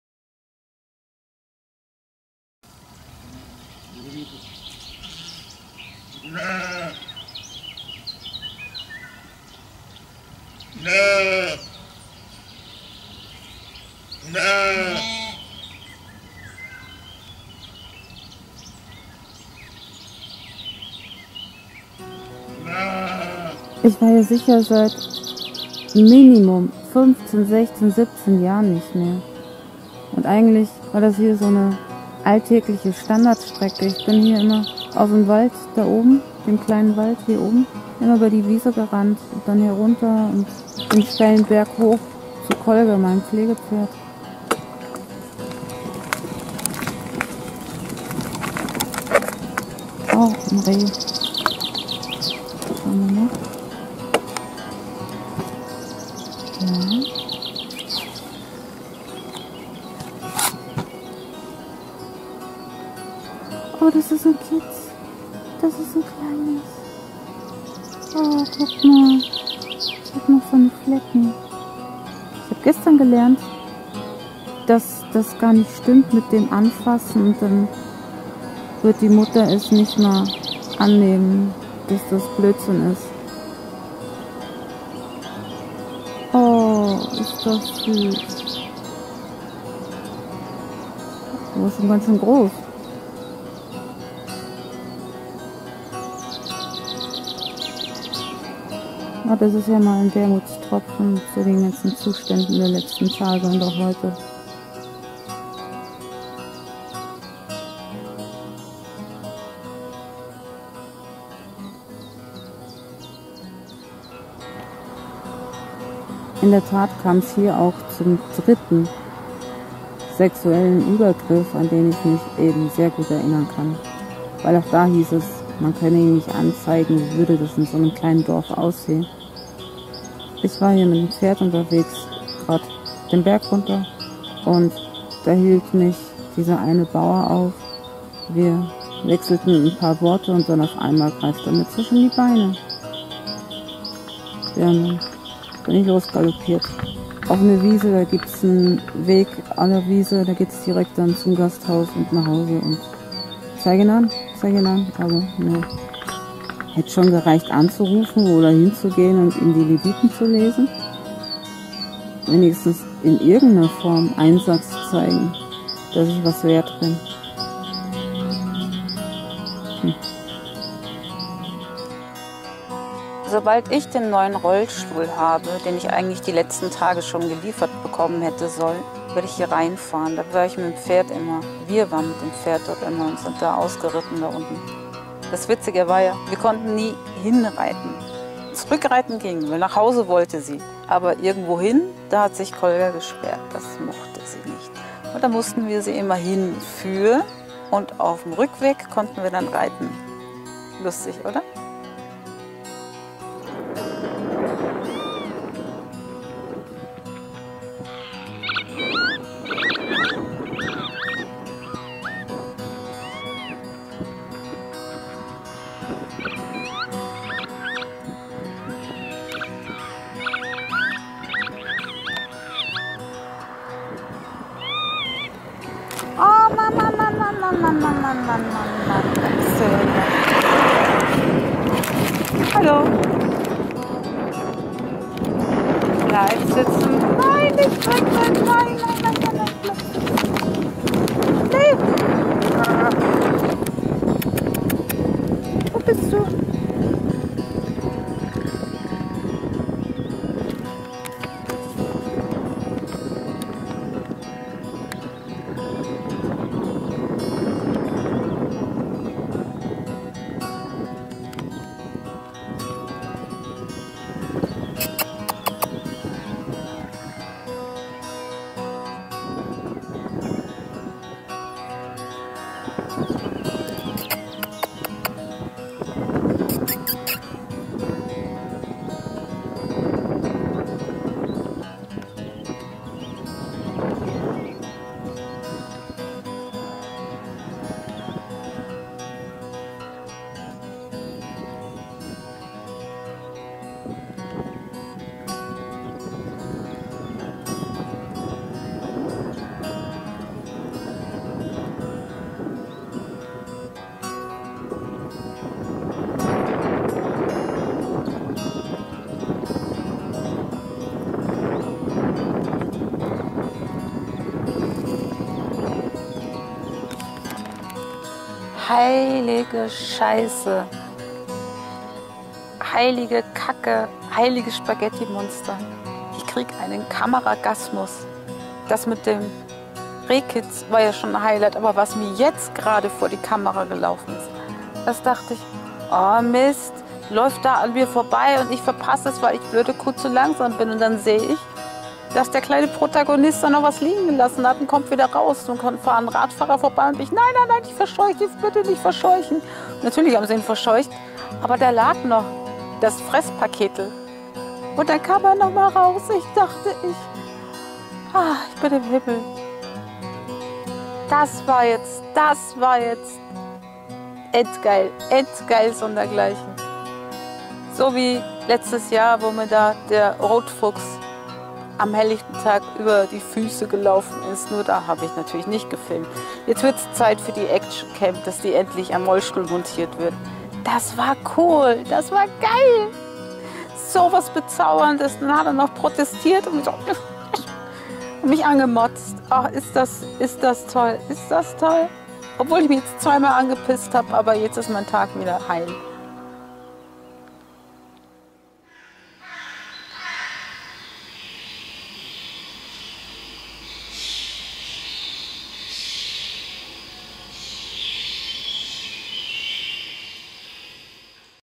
Tödlich niedlich - entzückende Tiere entdecken, ein Tag voller Überraschungen!!
Kommt mit mir auf eine kurze Safari durchs Henndorf Hügelland, um selten gesehene Tiere zu beobachten. Was mir an dem Tag alles vor die Linse gelaufen ist… Es ist an sich kinderfreundlich, aber es geht dennoch auch um sexuelle Übergriffe. Also hütet euch, es wird unglaublich niedlich!!

Multiple vs Multiple #44

Na? Schaff' ich die 24?